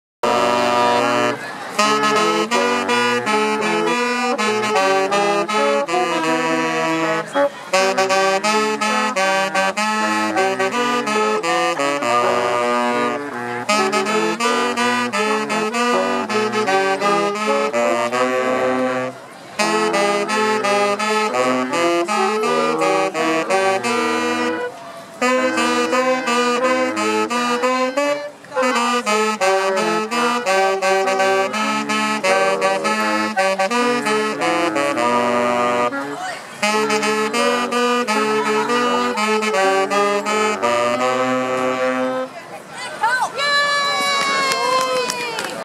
Geneva Band Students Play At The 2019 Christmas Walk
Geneva High School students delighted patrons as they strolled Downtown Geneva during the Christmas Walk festival Dec. 6, 2019.